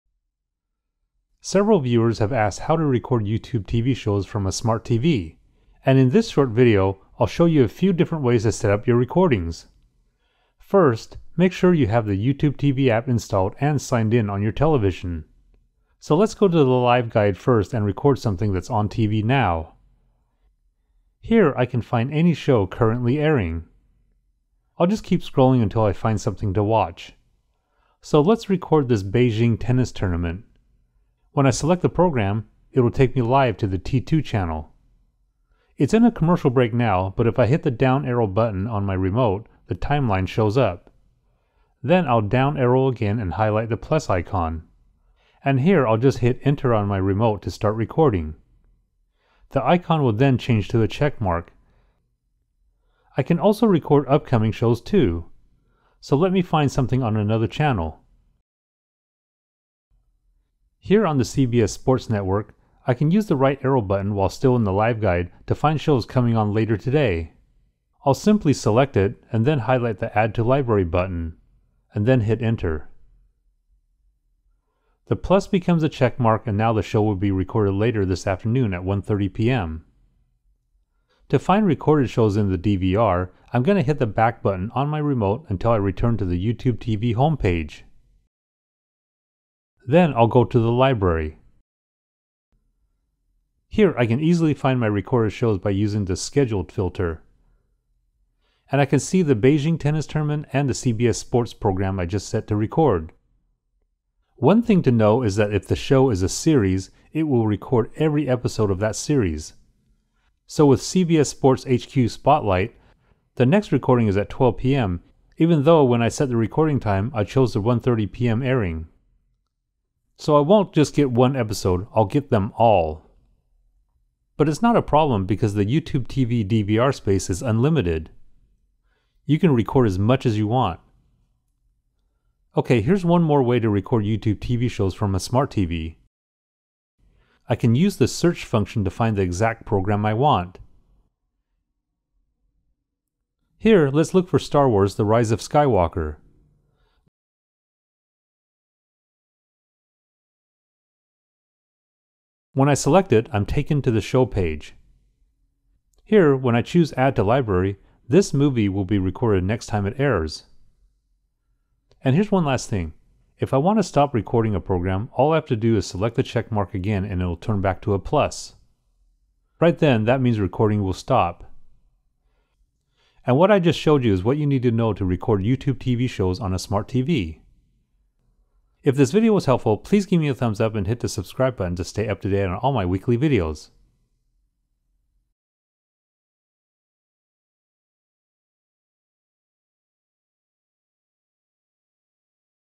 How to Record YouTube TV Shows on a Smart TV - How to Record on YouTube TV DVR
Product Links:
Roku with YouTube TV
Chromecast with YouTube TV

Chapters:
00:00 - YouTube TV Recording Introduction
00:19 - Record Shows From Live Guide
01:00 - Record Future Broadcasts
01:19 - Find Recorded Shows in the DVR
02:16 - Searching for Shows to Record

Several viewers have asked how to record YouTube TV shows from a smart TV. And in this short video, I'll show you a few different ways to set up your recordings. First, make sure you have the YouTube TV app installed and signed in on your television. I'll explain through the Live Guide first and record something that is on TV now. Here I can find any show currently airing. I'm going to record a Beijing tennis tournament. When I select the program it'll take me live to the T2 channel. While the show is playing, if I hit the down arrow button on my remote, the timeline shows up. Then I'll down arrow again and highlight the Plus icon. And I'll just hit Enter in my remote to start recording. The icon will then change to a check mark.

I can also record upcoming shows too. In the example, I found the CBS Sports Network and I can use the right arrow button while still in the Live Guide to find shows coming on later today. I'll simply select it and then highlight the Add to Library button and then hit Enter. The Plus becomes a check mark and now the show will be recorded later this afternoon at 1:30pm.

To find recorded shows in the DVR, I'm going to hit the back button on my remote until I return to the YouTube TV homepage. Then I'll go to the Library. Here I can easily find my recorded show by using the Scheduled filter. And I can see the shows I set to record. One thing to know is that if the show is a series, it will record every episode of that series. So with CBS Sports HQ Spotlight, the next recording is at 12pm even though when I set the recording time, I chose the 1:30pm airing. So I won't get just one episode, I'll get them all. But it's not a problem because the YouTube TV DVR space is unlimited. You can record as much as you want.

Here's one more way to record YouTube TV shows from a smart TV. I can use the Search function to find the exact program I want. Here I look for Star Wars: The Rise of Skywalker. When I select it, I'm taken to the show page. Here when I choose Add to Library, this movie will be recorded next time it airs. And here's one last thing, if I want to stop recording a program, all I have to do is select the check mark again and it'll turn back into a Plus. Right then, that means recording will stop. That's what you need to know to record YouTube TV shows on a smart TV.